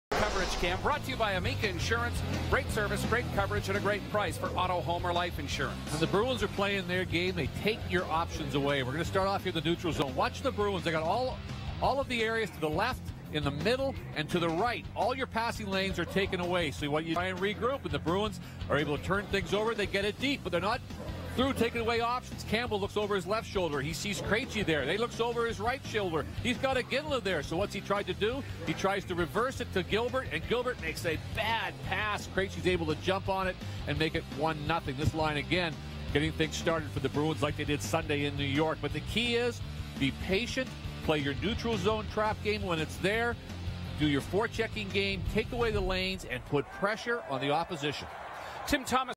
Gregory Campbell's Forechecking Leads To David Krejci Goal
The Bruins get on the board first against the Florida Panthers.